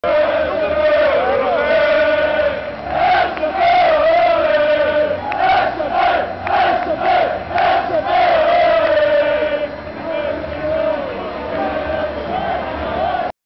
sfeer vak svzw
sfeer na de overwinning in beker van belgie op standard, befaamde 'segios truitje had last van de wind ' match